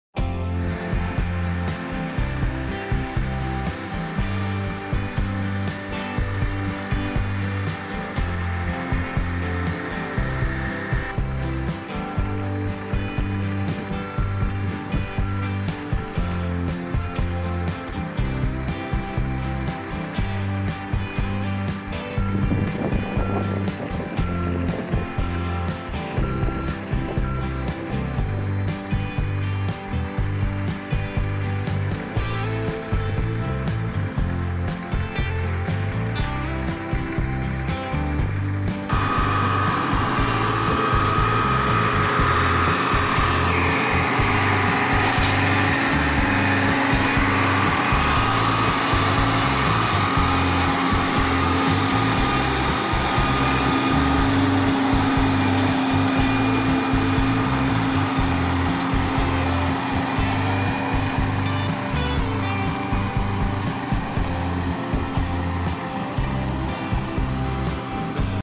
Chip seal up close
See how a county road chip seal is done.